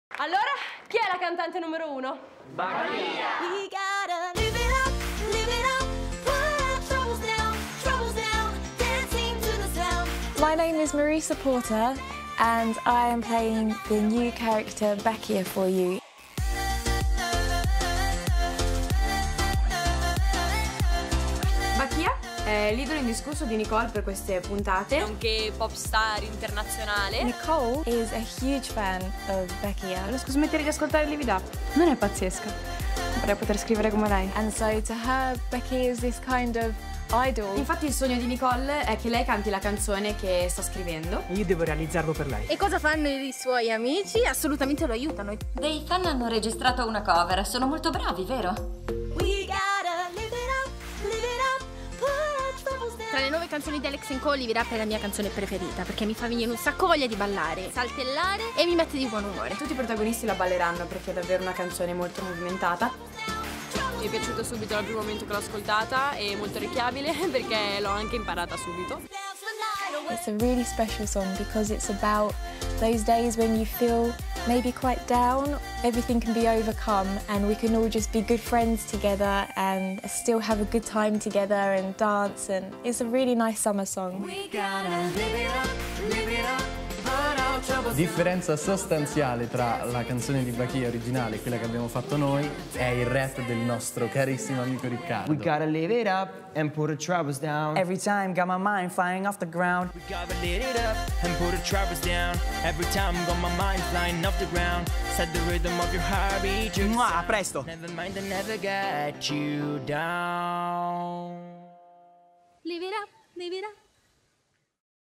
Backstage episodi nuovi Alex e co...conosciamo meglio i nuovi personaggi!
MUSICALLY:giuliatorrigiani
In questo video non saluto saluterò nel prossimo Perché non posso editarlo
Vi ricordo che da oggi ci sono i 4 episodi di Alex&co. Non perdeteli! Solo su Disney channel❤❤❤❤